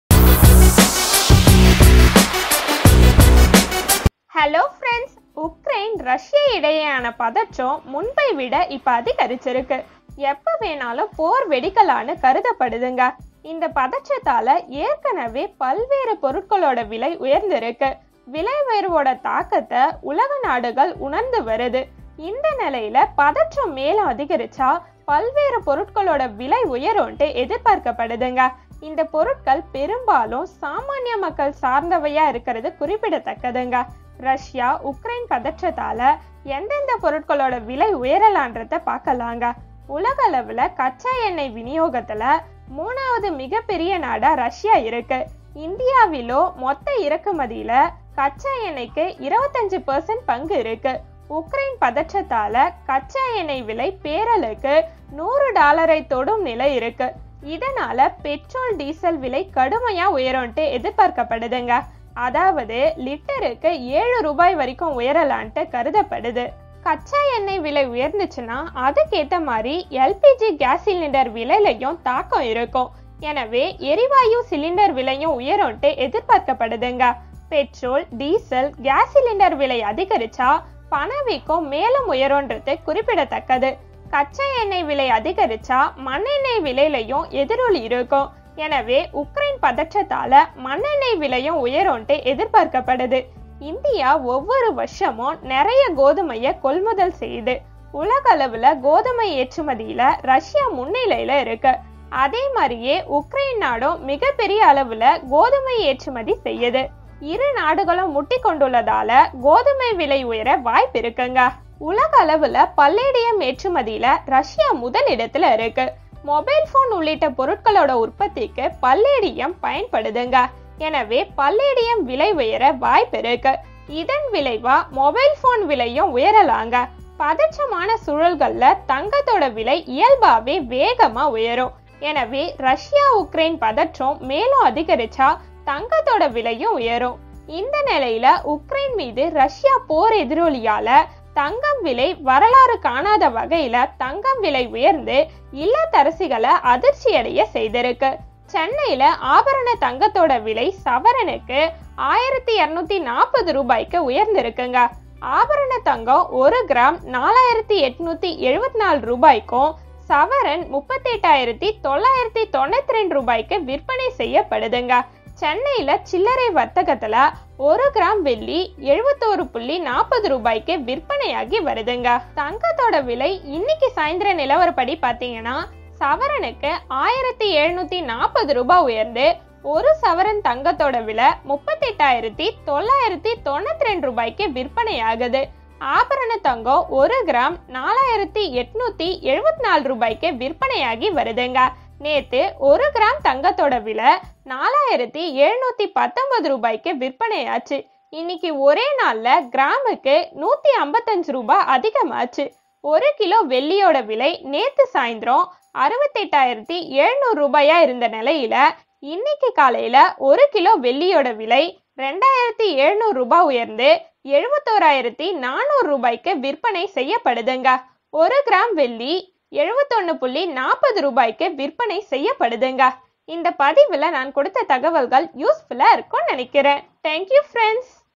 கேஸ் சிலிண்டர் முதல் பெட்ரோல் வரை🔥என்னெல்லாம் விலை உயரப்போகுது?🔥மக்களை பதம் பார்க்கப்போகும் விலைவாசி
🔥கேஸ் சிலிண்டர் முதல் பெட்ரோல் வரை.. என்னெல்லாம் விலை உயரப்போகுது தெரியுமா?🔥 சாமானிய மக்களை பதம் பார்க்கப்போகும் விலை வாசி🔥

உக்ரைன், ரஷ்யா இடையேயான பதற்றம் முன்பை விட இப்போது அதிகரித்துள்ளது. எப்போது வேண்டுமானாலும் போர் வெடிக்கலாம் என கருதப்படுகிறது. இந்த பதற்றத்தால் ஏற்கெனவே பல்வேறு பொருட்களின் விலை உயர்ந்துள்ளது. விலை உயர்வின் தாக்கத்தை உலக நாடுகள் உணர்ந்து வருகின்றன.

இந்நிலையில், பதற்றம் மேலும் அதிகரித்தால் பல்வேறு பொருட்களின் விலை உயரும் என எதிர்பார்க்கப்படுகிறது. இப்பொருட்கள் பெரும்பாலும் சாமானிய மக்கள் சார்ந்தவையாக இருப்பது குறிப்பிடத்தக்கது. ரஷ்யா - உக்ரைன் பதற்றத்தால் எந்தெந்த பொருட்களின் விலை உயரலாம் என்பதை பார்ப்போம்.
உலகளவில் கச்சா எண்ணெய் விநியோகத்தில் மூன்றாவது மிகப்பெரிய நாடாக ரஷ்யா உள்ளது. இந்தியாவிலோ மொத்த இறக்குமதியில் கச்சா எண்ணெய்க்கு 25% பங்கு உள்ளது. உக்ரைன் பதற்றத்தால் கச்சா எண்ணெய் விலை பேரலுக்கு 100 டாலரை தொடும் நிலையில் உள்ளது. இதனால், பெட்ரோல், டீசல் விலை கடுமையாக உயரும் என எதிர்பார்க்கப்படுகிறது. அதாவது லிட்டருக்கு 7 ரூபாய் வரை உயரலாம்  என கருதப்படுகிறது.

கச்சா எண்ணெய் விலை உயர்ந்தால் அதற்கு ஏற்ப எல்பிஜி கேஸ் சிலிண்டர் விலையிலும் தாக்கம் இருக்கும். எனவே எரிவாயு சிலிண்டர் விலையும் உயரும் என எதிர்பார்க்கப்படுகிறது. பெட்ரோல், டீசல், கேஸ் சிலிண்டர் விலை உயர்ந்தால் பணவீக்கம் மேலும் உயரும் என்பது குறிப்பிடத்தக்கது.
 கச்சா எண்ணெய் விலை உயர்ந்தால் மண்ணெண்ணெய் விலையிலும் எதிரொலி இருக்கும். எனவே, உக்ரைன் பதற்றத்தால் மண்ணெண்ணெய் விலையும் உயரும் என எதிர்பார்க்கப்படுகிறது.

இந்தியா ஒவ்வொரு ஆண்டும் நிறைய கோதுமையை கொள்முதல் செய்கிறது. உலகளவில் கோதுமை ஏற்றுமதியில் ரஷ்யா முன்னிலையில் உள்ளது. அதேபோல, உக்ரைன் நாடும் மிகப்பெரிய அளவில் கோதுமை ஏற்றுமதி செய்கிறது. இரு நாடுகளும் முட்டிக்கொண்டுள்ளதால் கோதுமை விலை உயர வாய்ப்புள்ளது.

உலகளவில் பல்லேடியம் (Palladium) ஏற்றுமதியில் ரஷ்யா முதலிடத்தில் உள்ளது. மொபைல் போன் உள்ளிட்ட பொருட்களின் உற்பத்திக்கு பல்லேடியம் பயன்படுகிறது. எனவே, பல்லேடியம் விலை உயர வாய்ப்புள்ளது. இதன் விளைவாக மொபைல் போன் விலையும் உயரலாம். பதற்றமான சூழல்களில் தங்கத்தின் விலை இயல்பாகவே வேகமாக உயரும். எனவே, ரஷ்யா - உக்ரைன் பதற்றம் மேலும் அதிகரித்தால் தங்கத்தின் விலையும் உயரும்.
இந்நிலையில், உக்ரைன் மீது ரஷ்யா போர் எதிரொலியால் தங்கம் விலை வரலாறு காணாத வகையில் தங்கம் விலை உயர்ந்து இல்லத்தரசிகளை அதிர்ச்சியடைய செய்துள்ளது.
சென்னையில் ஆபரணத் தங்கத்தின் விலை சவரனுக்கு ரூ.1,240 உயர்ந்துள்ளது. ஆபரணத் தங்கம் ஒரு கிராம் ரூ.4,874 க்கும், சவரன் ரூ.38,992-க்கு விற்பனை செய்யப்படுகிறது. சென்னையில் சில்லறை வர்க்கத்தில் ஒரு கிராம் வெள்ளி ரூ.71.40-க்கு விற்பனையாகி வருகிறது.
தங்கத்தின் விலை இன்று மாலை நிலவரப்படி சவரனுக்கு 1,240 ரூபாய் உயர்ந்து ஒரு சவரன் தங்கத்தின் விலை ரூ.38,992-க்கு விற்பனையாகிறது. ஆபரணத் தங்கம் ஒரு கிராம் ரூ.4,874 விற்பனையாகி வருகிறது. நேற்று ஒரு கிராம் தங்கத்தின் விலை 4,719 க்கு விற்பனையானது. இன்று ஒரே நாளில் கிராமுக்கு 155 ரூபாய் அதிகமானது.

ஒரு கிலோ வெள்ளியின் விலை நேற்று மாலை 68,700 ரூபாயாக இருந்த நிலையில், இன்று காலையில் வெள்ளியின் விலை கிலோவுக்கு 2,700 ரூபாய் உயர்ந்து  ரூ.71,400-க்கு விற்பனை செய்யப்படுகின்றன. ஒரு கிராம் வெள்ளி ரூ.71.40-க்கு விற்பனை செய்யப்படுகிறது.